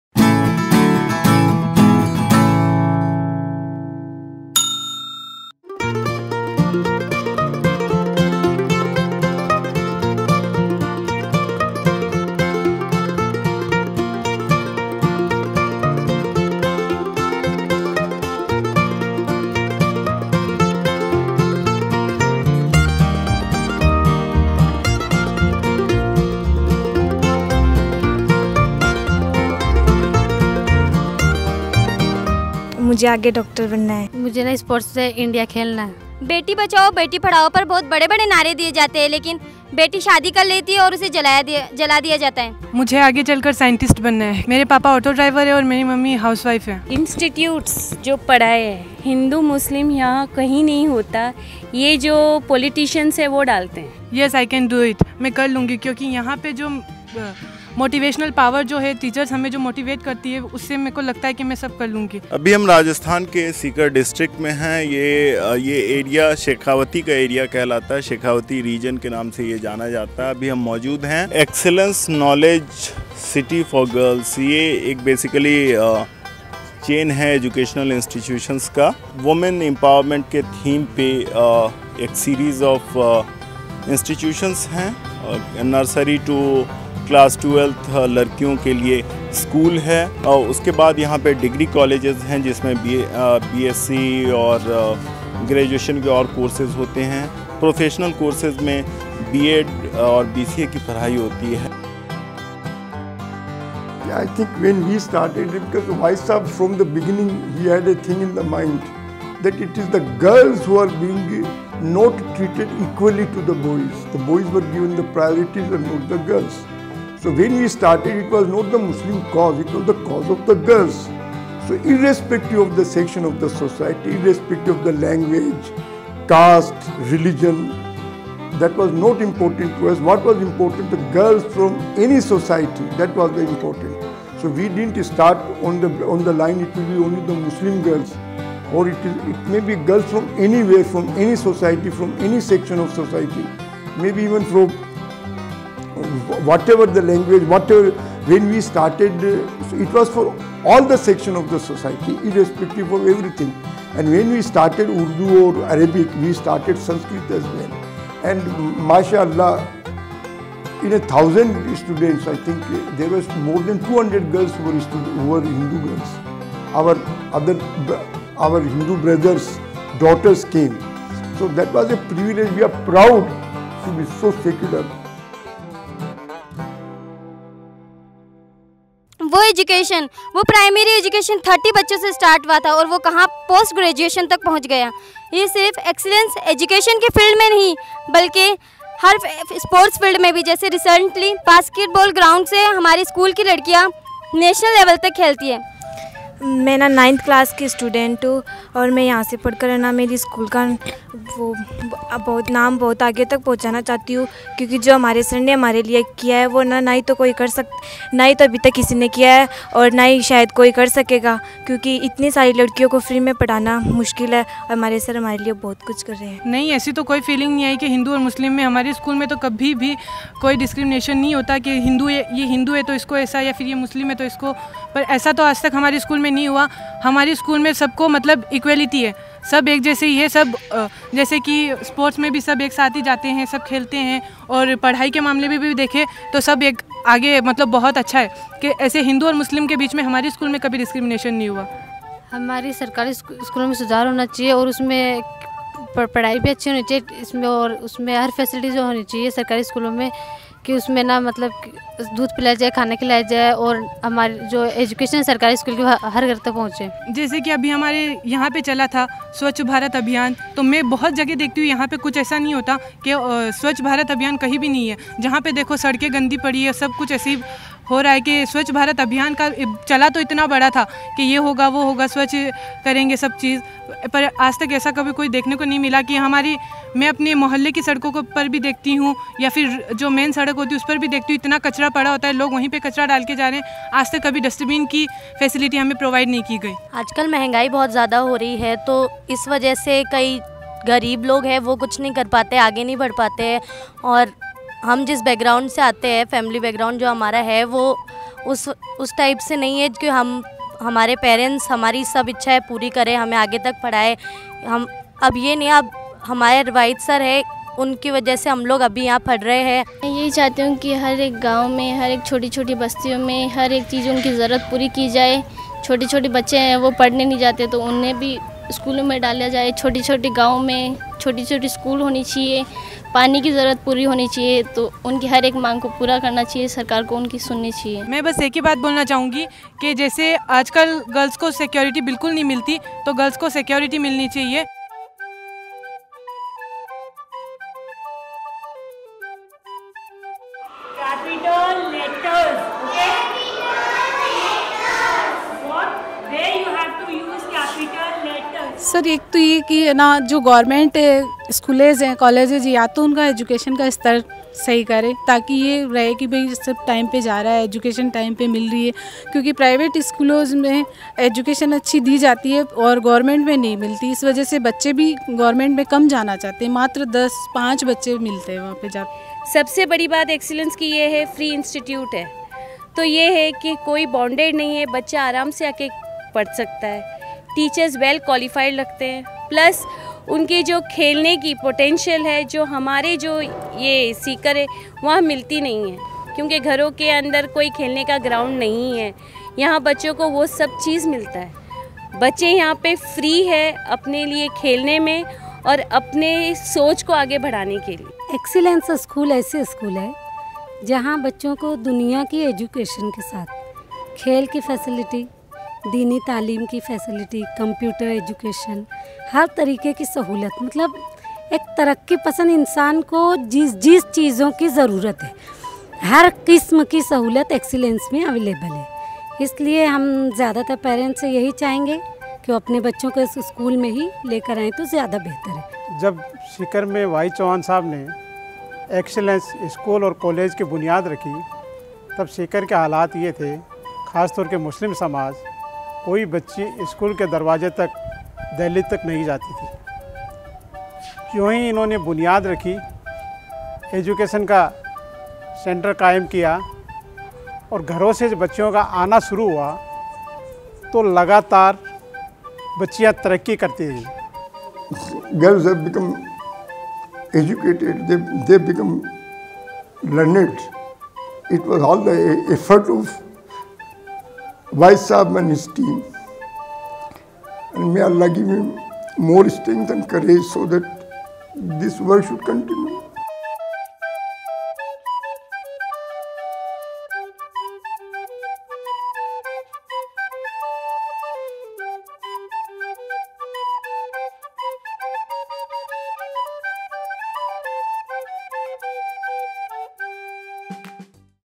Excellence Knowledge City: An Oasis for Education of Underprivileged Girls in Rajasthan
Excellence Knowledge City for Girls, an oasis for girls' education in Sikar district of the desert state Rajasthan, offers free education along with books and uniform.
The institution, which offers education from nursery to B.Sc and B.Ed. to underprivileged girls in the Shekhawati region, runs on entirely savings of entrepreneur Wahid Chowhan who was born in Sikar, and is now settled in Mumbai.
This is probably the first institution that teaches Sanskrit along with Urdu and Arabic. It has completed 20 years successfully.